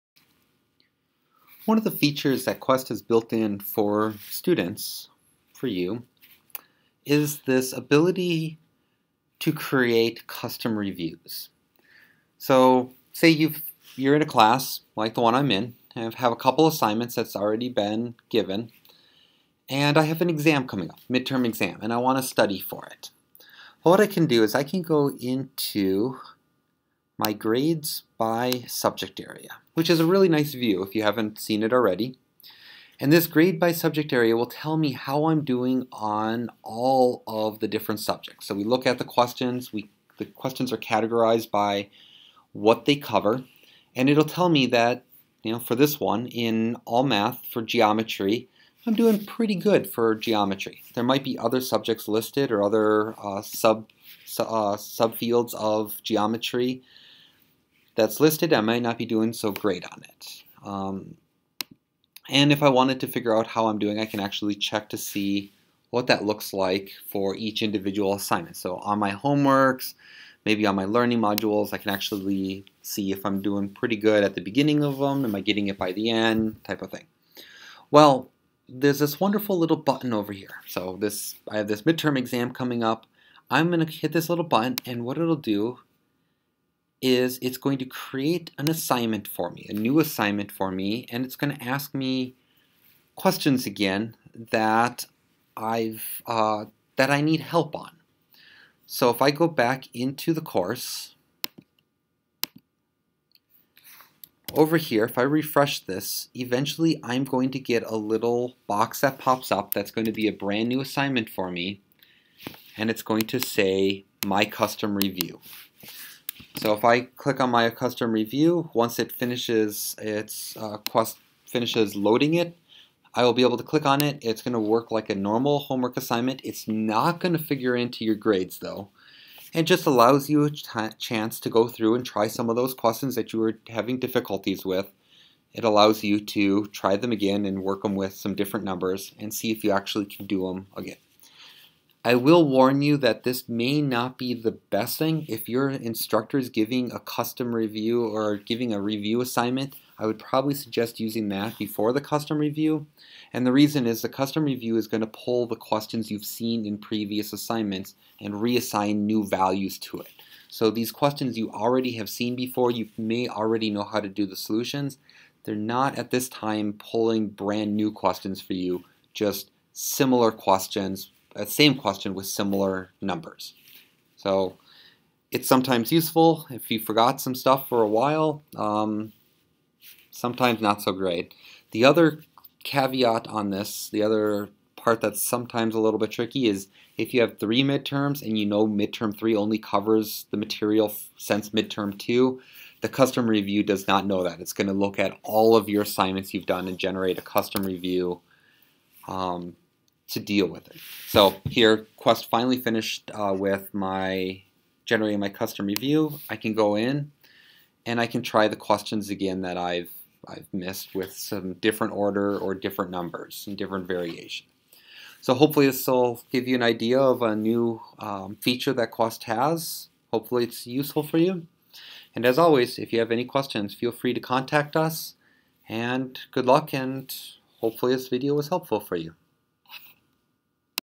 Custom Reviews
This video will cover how a student can generate a custom review tailored to their needs. Hosted by Nathan W Erickson, PhD